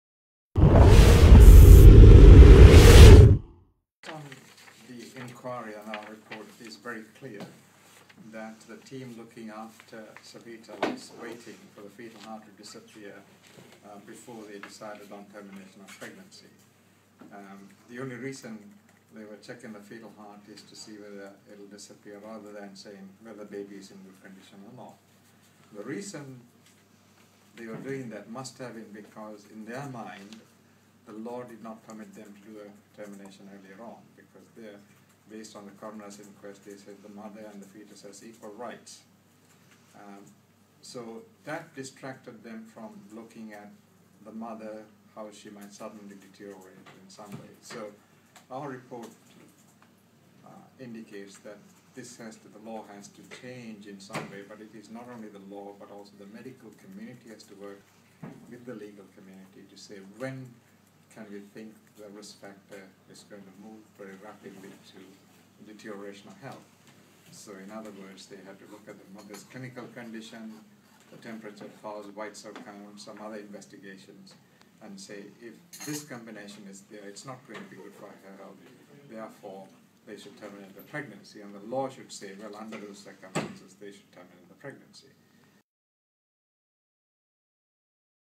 HSE report: Legal uncertainty contributed to Savita's death
Dr. Sabaratnam Arulkumaran says the medical community - as well the law - needs to change